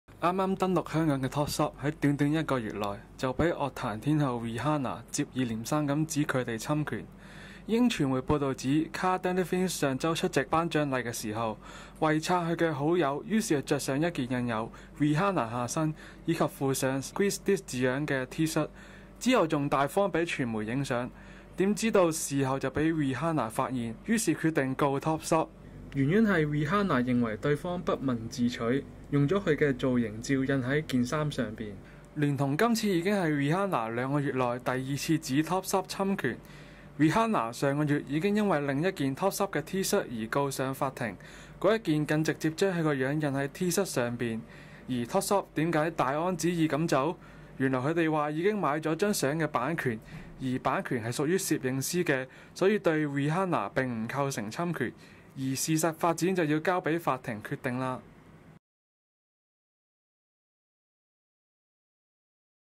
[HKipnews] Rihanna告Topshop侵權
樂壇天后Rihanna日前指Topshop未經其授權，擅自將印有其性感臀部的-shirt發售，其名模好友Cara Deevngne更在不知情下更穿上該「侵權T-shirt」出席頒獎禮，使Rihanna憤怒不已，決定二度告上法庭。(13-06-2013)